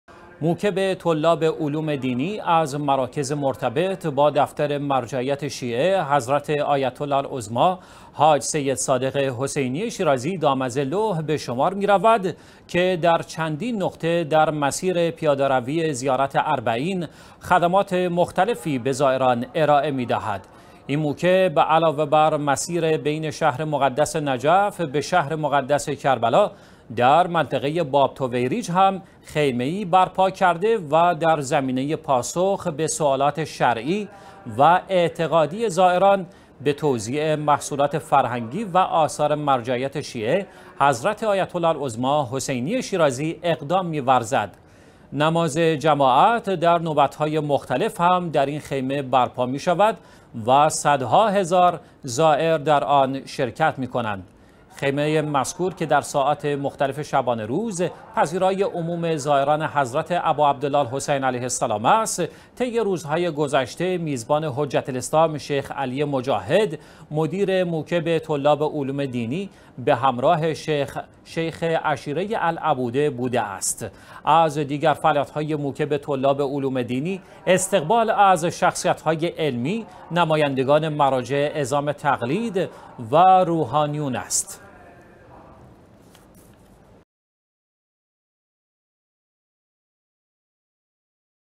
فعالیت موکب طلاب علوم دینی در مسیر زائران پیاده رویی اربعین
موکب طلاب علوم دینی، از مراکز مرتبط با دفتر مرجعیت شیعه، حضرت آیت الله العظمی حاج سید "صادق حسینی شیرازی" دام ظله به شمار می رود که در چندین نقطه در مسیر پیاده روی زیارت اربعین، خدمات مختلفی به زائران ارائه می‌دهد.
این موکب علاوه بر مسیر بین شهر مقدس نجف به شهر مقدس کربلا، در منطقه باب طویریج هم خیمه ای برپا کرده و در زمینه پاسخ به سوالات شرعی و اعتقادی زائران، به توزیع محصولات فرهنگی و آثار مرجعیت شیعه، حضرت آیت الله العظمی حسینی شیرازی، اقدام می ورزد. نماز جماعت در نوبت های مختلف هم در این خمیه برپا می شود و صدها زائر در آن شرکت می کنند.
خیمه مذکور که در ساعات مختلف شبانه روز پذیرای عموم زائران حضرت اباعبدالله الحسین علیه السلام است، طی روزهای گذشته میزبان حجت الاسلام شیخ "علی مجاهد" مدیر موکب طلاب علوم دینی به همراه شیخ "عشیره العبوده" بوده است.
از دیگر فعالیت های موکب طلاب علوم دینی، استقبال از شخصیت های علمی، نمایندگان مراجع عظام تقلید و روحانیان است.